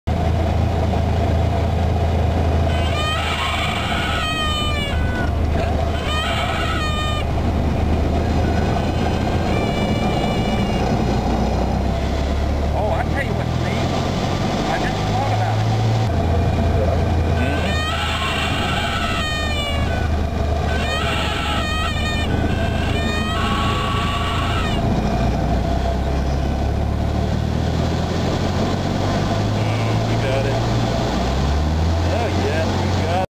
Spin_right_Nov012010.mpg
Right spin video from outside and inside the plane. Cessna 150 Aerobat. N6146F